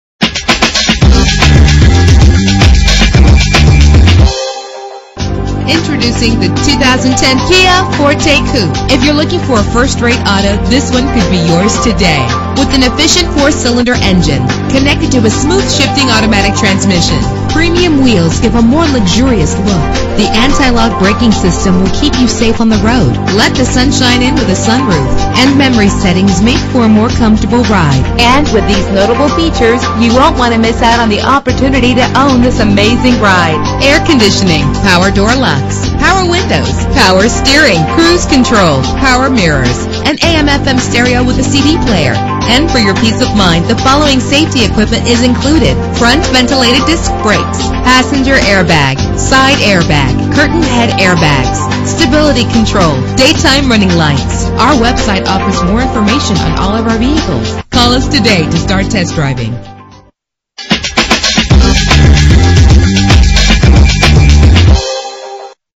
2010 Kia Forte Koup - Sanford FL
Year: 2010
Make: Kia
Model: Forte Koup
Trim: SX
Engine: 2.4 liter inline 4 cylinder 16 valve
Transmission: Automatic
Color: Blue
Mileage: 70734
Address:
1160 Rinehart Road